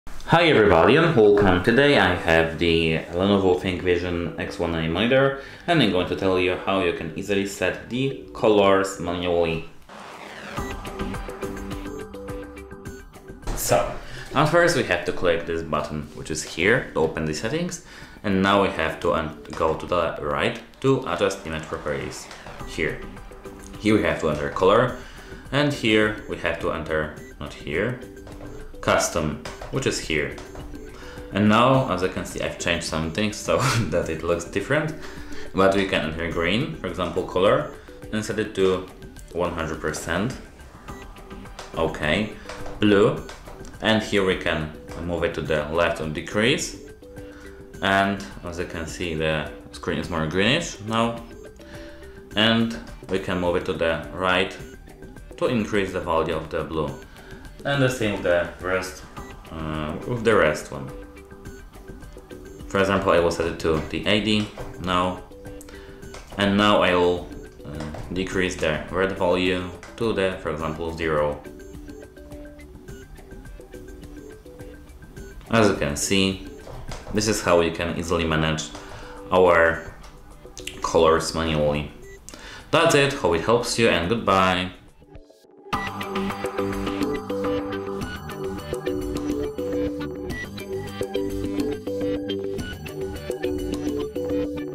LENOVO ThinkVision X1A – How to Set Monitor Colors Manually
If you want to adjust the color settings on your LENOVO ThinkVision X1A monitor, this video will show you step-by-step how to set the monitor colors manually. Learn how to access the settings menu, navigate to the color adjustment options, and fine-tune the red, green, and blue values for a custom display experience. Whether you need more accurate colors for work or want to personalize your screen, this guide makes it easy to manage color settings on the LENOVO X1A. Improve your viewing experience by following these simple instructions for manual color calibration on your ThinkVision monitor.

How to set monitor colors manually on LENOVO ThinkVision X1A?
How to adjust RGB color values on LENOVO X1A monitor?
Where are the color settings on LENOVO ThinkVision X1A?

0:00 Introduction to LENOVO ThinkVision X1A
0:06 Accessing the monitor settings menu
0:18 Navigating to image properties
0:24 Entering the color adjustment section
0:30 Selecting custom color mode
0:39 Adjusting green, blue, and red values
1:19 Finalizing manual color settings